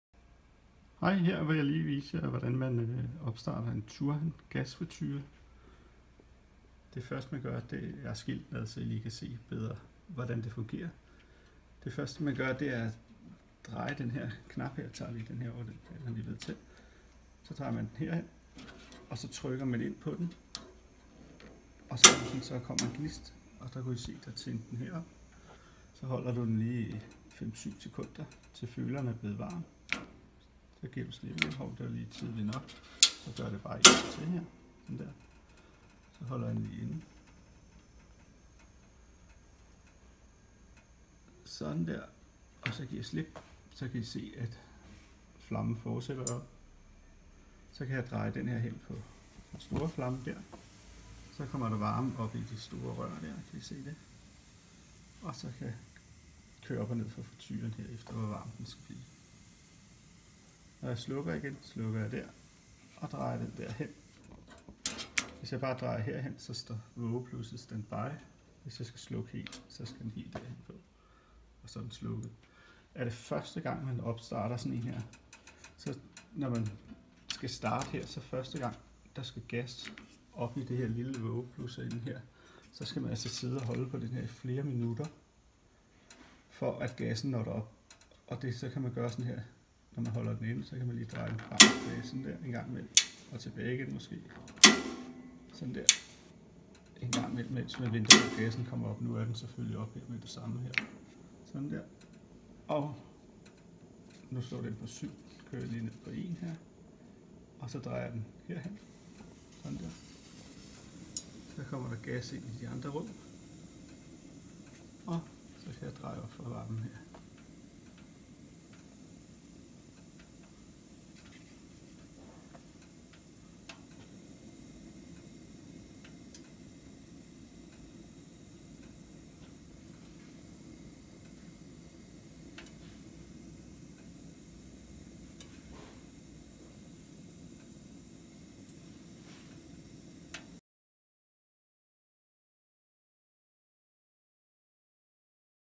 FastShop.dk - #0035 - Gas friture Turhan
Opstart af Gasfriture, se hvordan den ser ud indvendig.

Look inside a Gas Fryer.